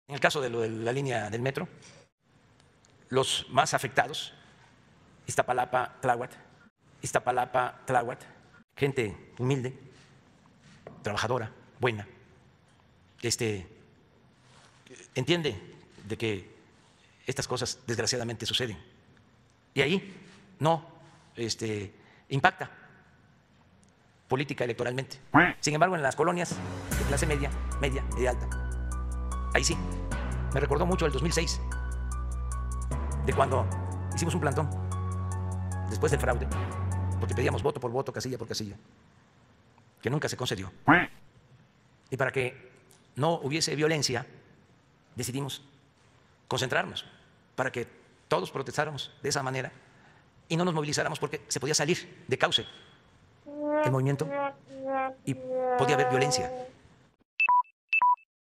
La gente humilde entiende que el metro se caiga, pero la clase media no: Andrés Manuel 🦆
El peor enemigo del Presidente es Andrés Manuel: en el caso de la Linea12DelMetro los más afectados Iztapalapa y Tláhuac "gente humilde, entiende que estas cosas desgraciadamente suceden" sin embargo, en las colonias de clase media ahí sí afecta electoralmente. 🦆